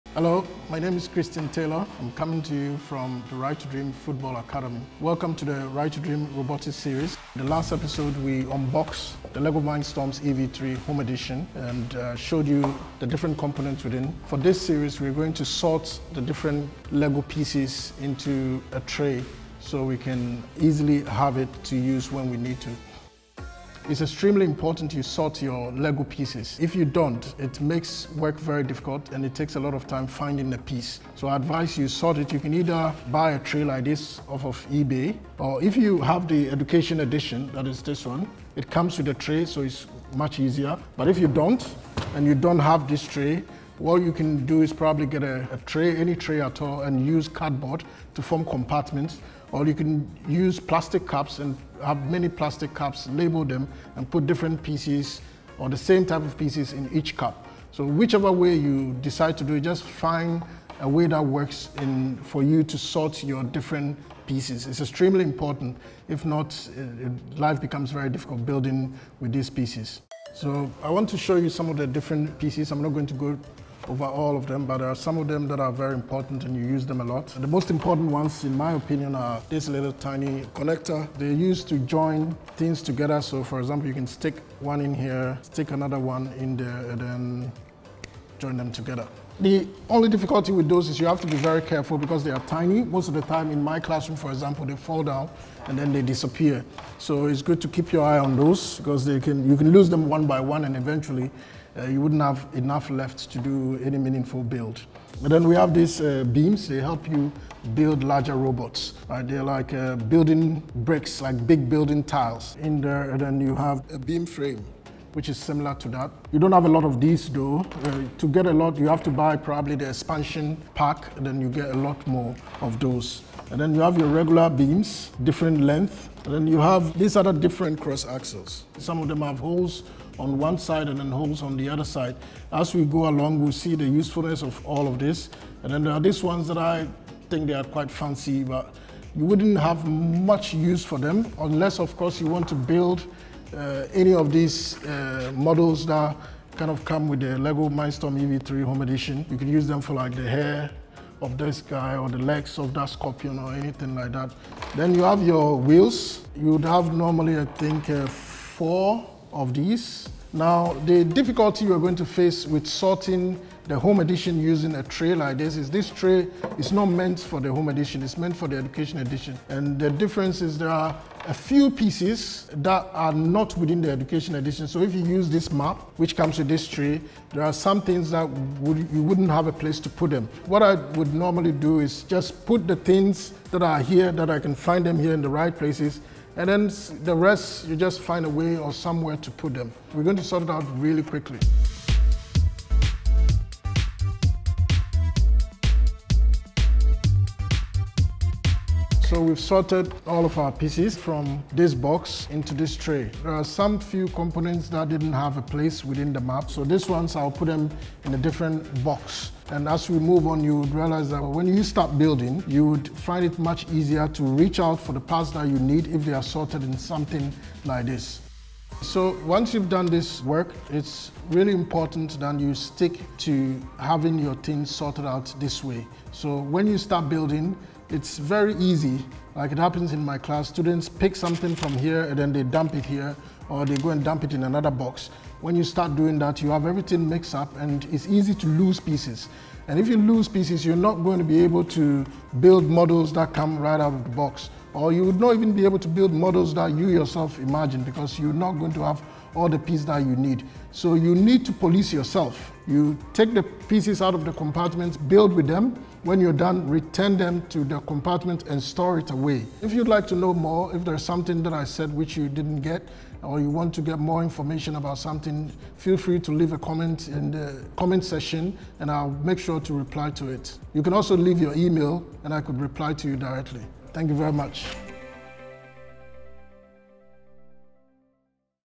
LEGO Mindstorms EV3 From Beginner to Expert – Sorting
If you’ve unboxed the Lego Mindstorms EV3 with us in the first episode (link), you’ll probably be a bit lost right now with all these tiny pieces, and apparently things can get a bit messy when you’re building a robot. Who knew?

Right to Dream’s STEM professor Christian Taylor tells us in this second episode about the best way to sort our Lego parts and introduces us to some of the most important pieces that will be our best companions in this exciting journey.